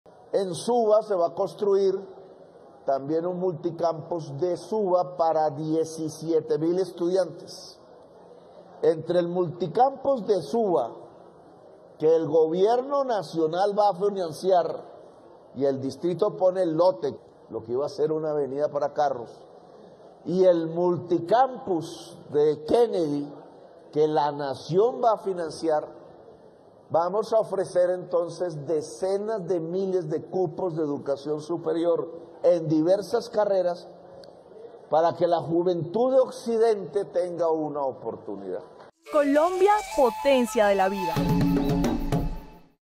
Anuncio del Presidente sobre la creación del multicampus de Suba y Kennedy
“Entre el multicampus de Suba y Kennedy que el GobiernoDelCambio va a financiar, vamos a ofrecer decenas de miles de cupos de#EducaciónSuperior para que la juventud del occidente de la capital tenga una oportunidad de estudiar”: presidente Gustavo Petro en GobiernoConElPueblo